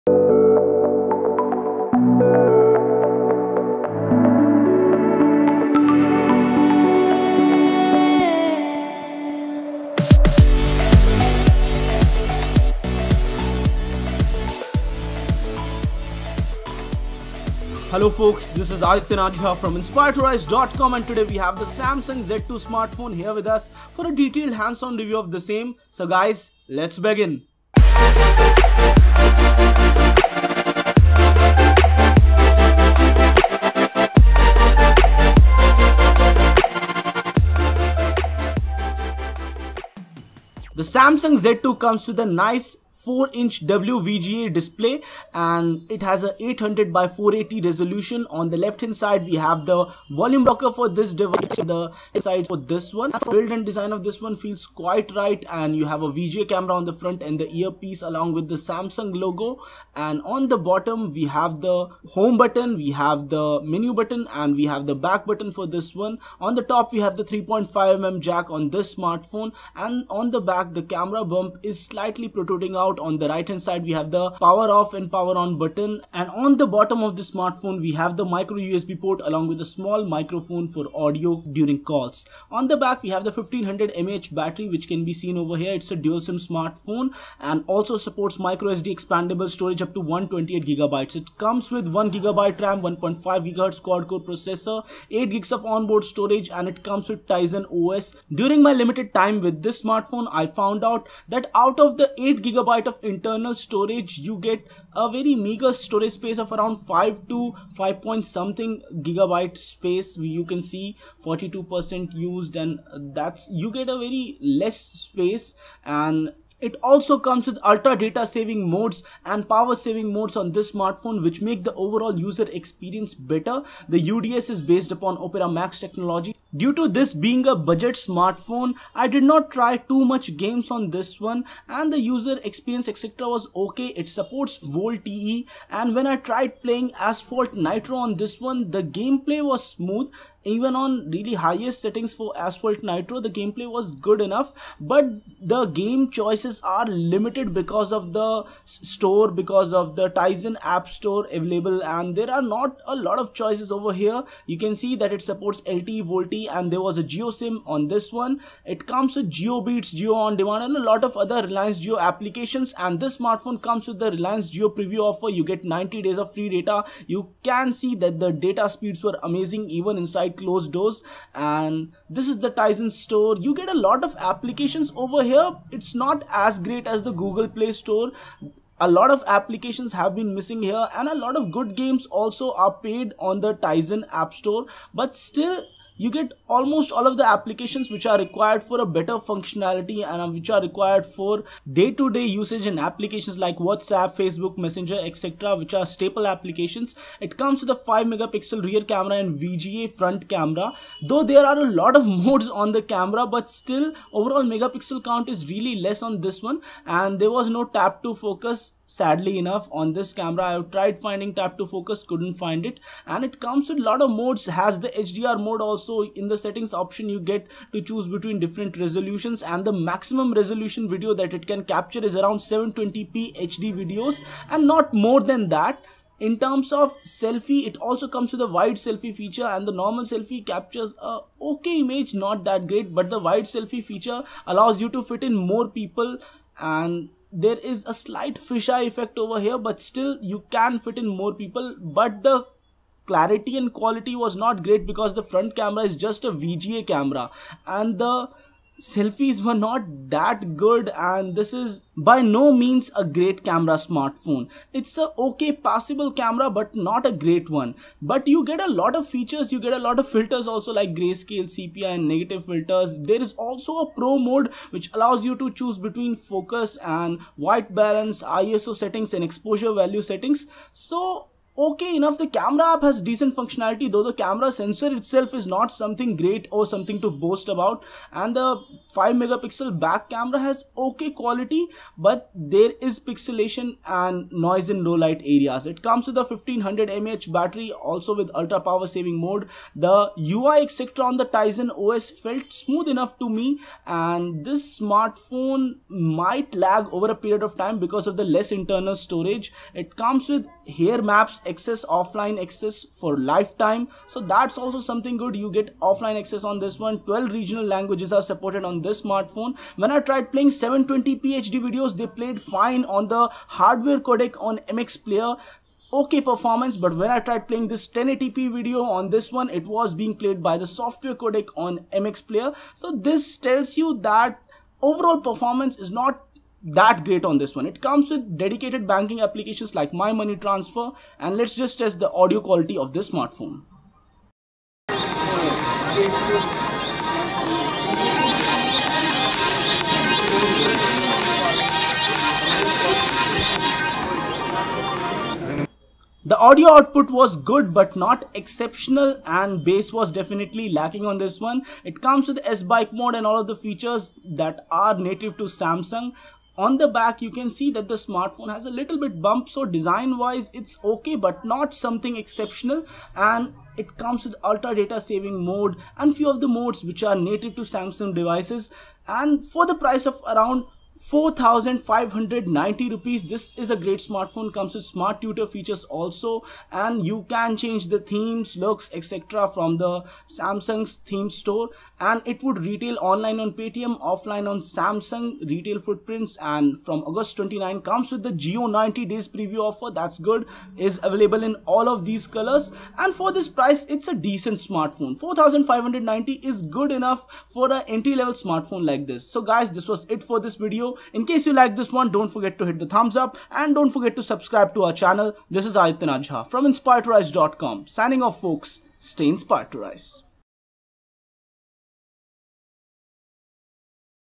Samsung Z2 hands on review (COMPLETE)
Samsung Z2 hands on review (COMPLETE): The Samsung Z2 is Samsung's latest budget smartphone offering, it comes with a 1.4 Ghz Quad Core processor, 1 GB of RAM, 8 GB of onboard storage with option to expand more upto 128 GB via a microSD card. The phone comes with Samsung's own Tizen OS and features all of the regular Tizen features like S-bike mode etc. It comes with 720p video recording on it's 5 MP primary camera and has a VGA front camera. At a price of INR 4950 it's a nice smartphone which offers decent functionality with 1500 mAH battery and a lot more!

//**Follow Inspire2rise**//

You were watching Samsung Z2 hands on review (COMPLETE) on Inspire2rise!